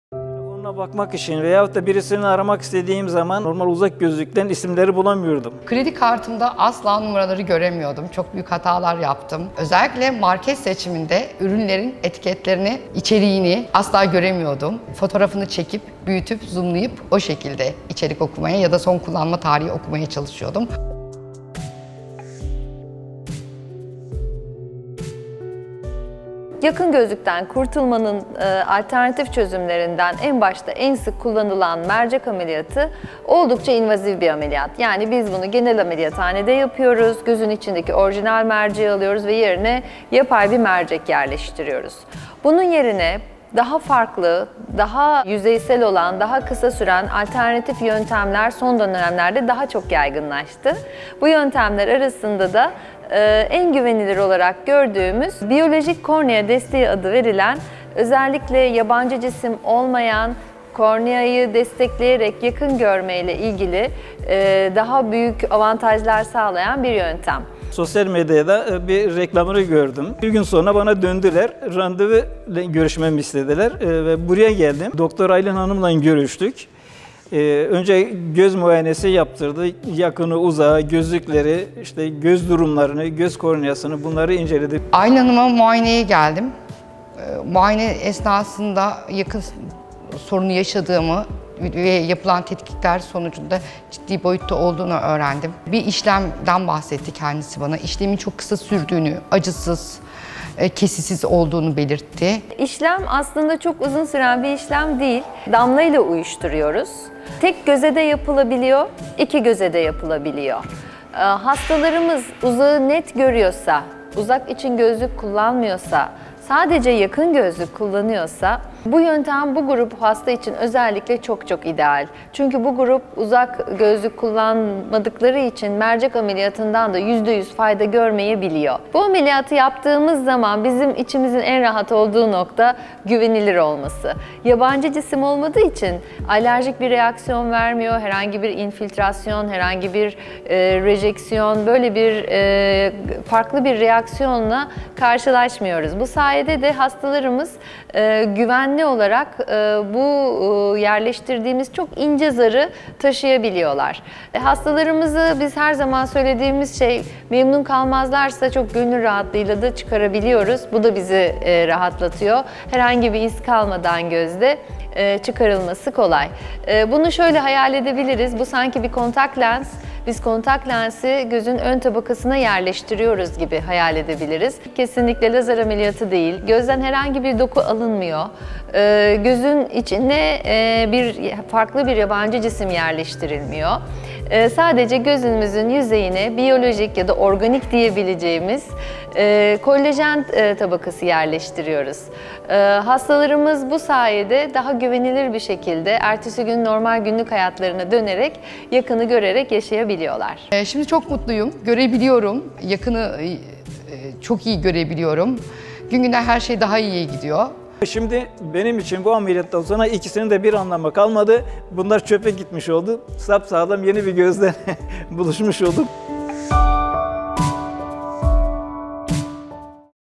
Medipol'de Uygulanan Biyolojik Kornea Desteğiyle Yakın Görme Problemlerinizden Kurtulmanız Mümkün
Türkiye’de sadece hastanemizde uygulanan Biyolojik Kornea Desteğiyle yakın görme problemlerinizden kurtulmanız mümkün.